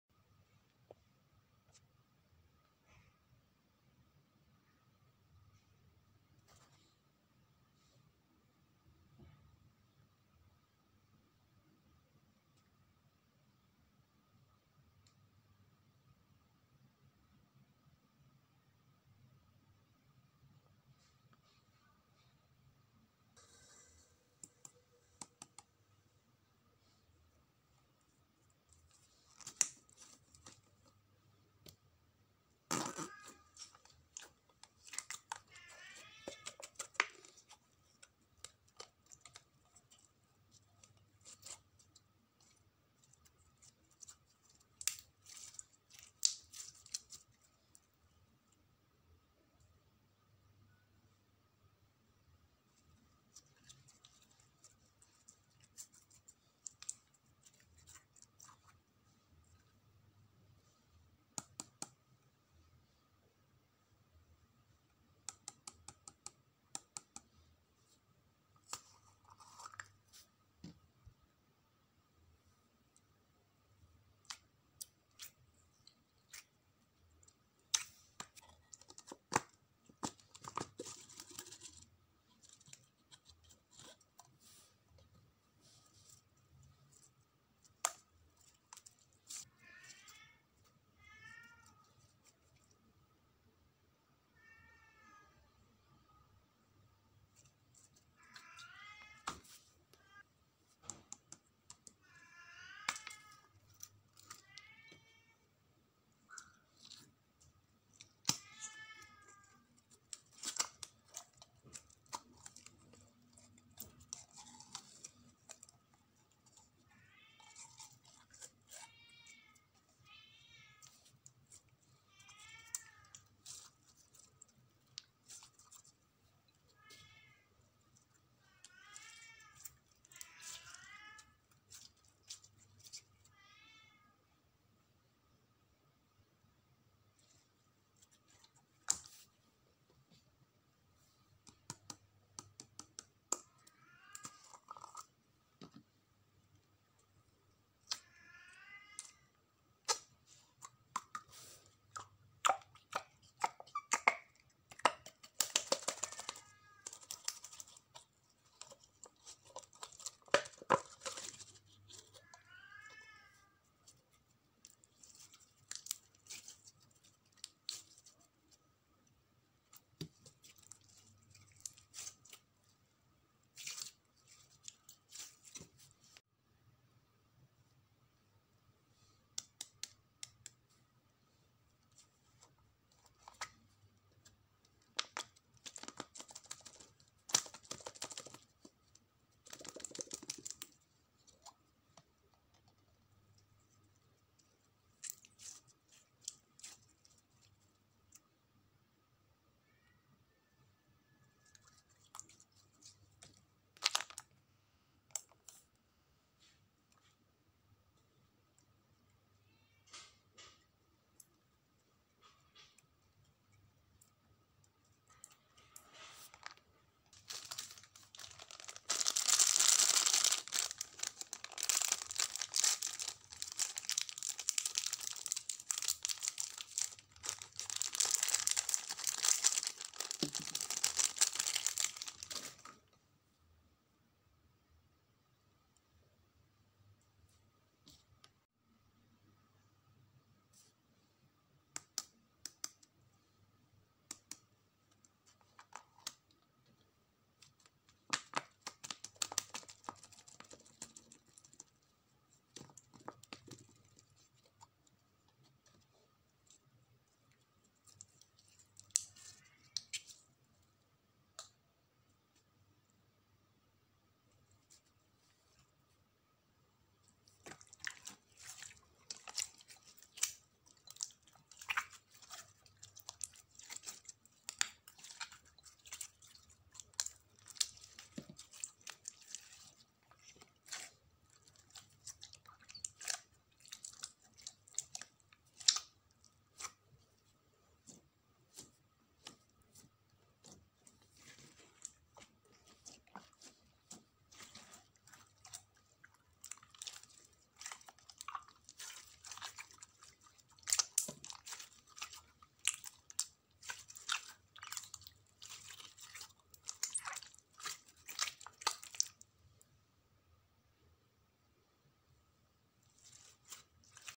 video slime yang lucu berkilau indah! beautiful shiny slime videos
video slime yang lucu berkilau indah

beautiful shiny slime videos

hai di video kali ini akan menggabungkan semua warna slime menjadi satu wadah

dan hasilnya sangat memuaskan dan indah